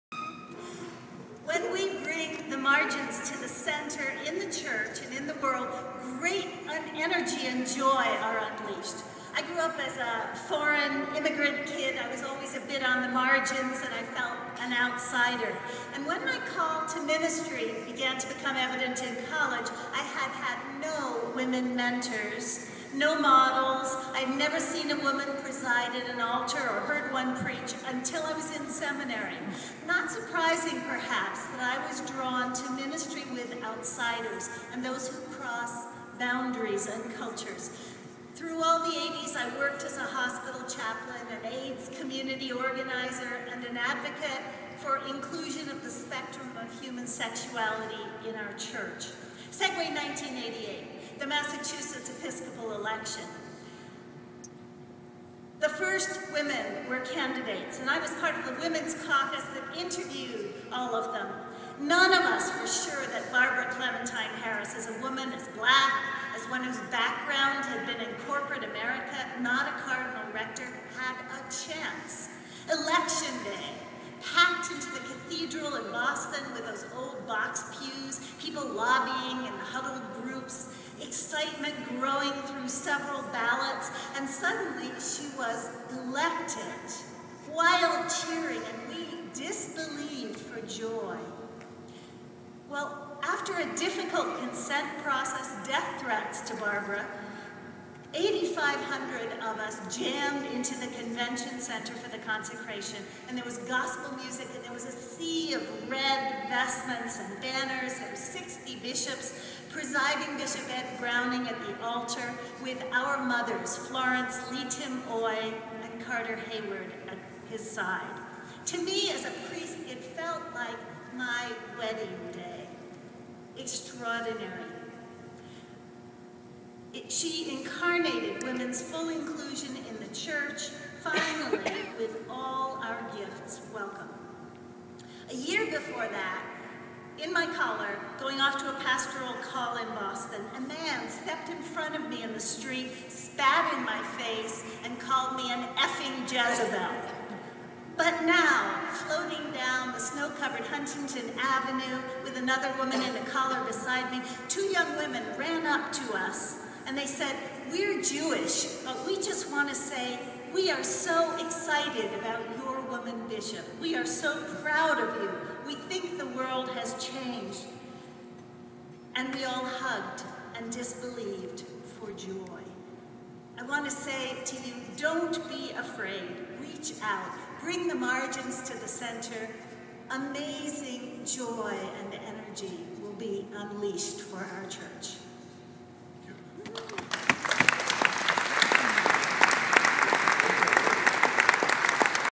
Jennifer Phillips at General Convention 09
The Rev. Jennifer Phillips offers a Public Narrative at the General Convention of the Episcopal Church. July 11, 2009.  This video was made with the knowledge and consent of Jennifer Phillips.